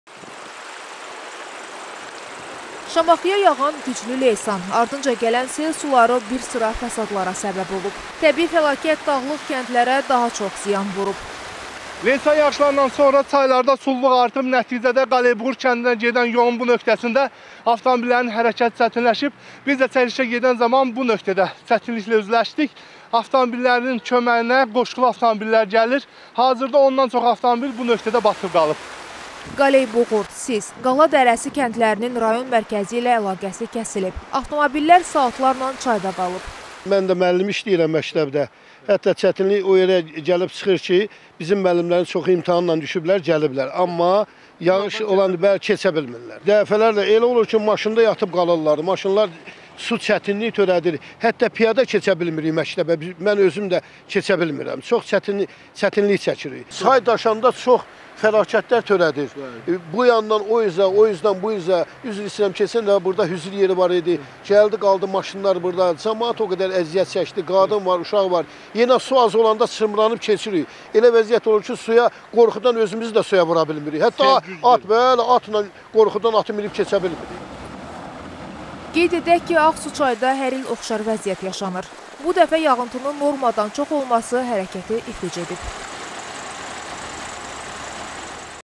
Üç kəndin Şamaxı ilə əlaqəsi kəsildi - BAKU TV
Şamaxıya yağan güclü leysan, ardınca gələn sel bir sıra fəsadlara səbəb olub.
Təbii fəlakət dağlıq kəndlərə daha çox ziyan vurub. Qaleybuğurd, Sis, Qaladərəsi kəndlərinin rayon mərkəzi ilə əlaqəsi kəsilib, avtomobillər saatlarla çayda qalıb.
Qeyd edək ki, Ağsuçayda hər il oxşar vəziyyət yaşanır. Bu dəfə yağıntının normadan çox olması hərəkəti iflic edib.

Üç kəndin Şamaxı ilə əlaqəsi kəsildi